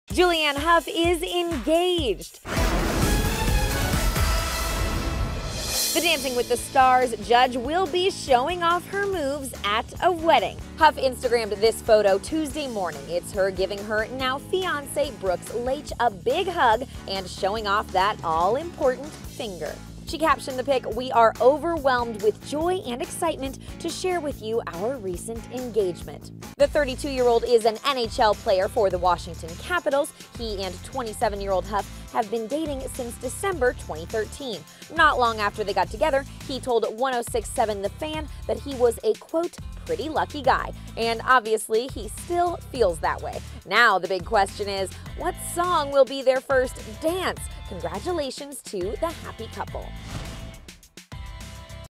Julianne Hough is Engaged to Brooks Laich! See the Pic
The 'Dancing With the Stars' judge now calls her hockey player beau her 'fiance.'